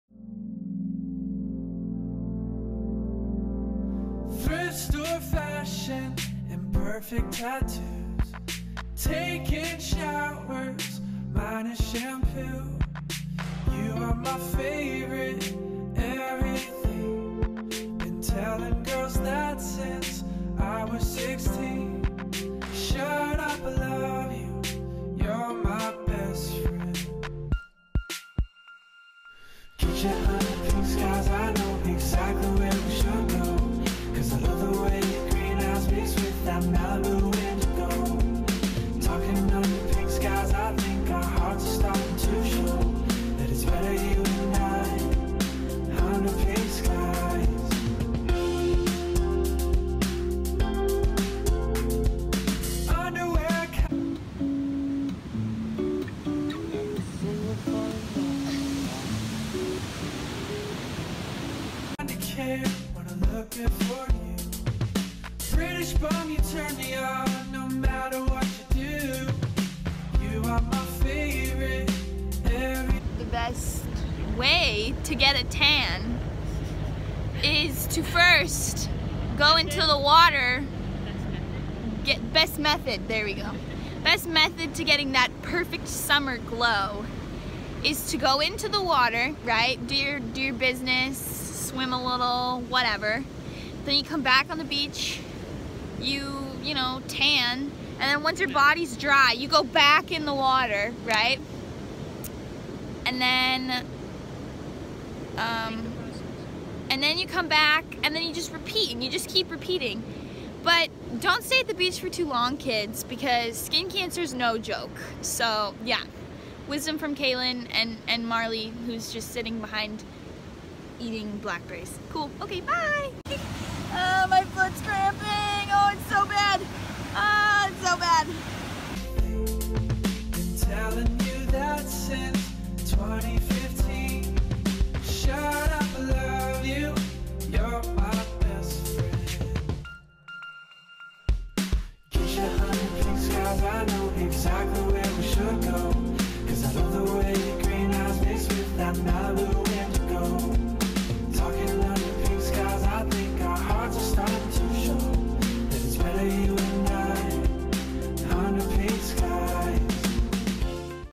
beach vlog - 15th street, del mar ca
i parked on 11th street (i knew free parking would be hard to find because its summer!) and we walked down the cliff at the end of the street. we just ended up walking over to the 15th street/powerhouse park area from 11th. del mar pizza is on 15th street and i put two dollar signs because its not cheap but not ridiculous ($4 dollars a slice)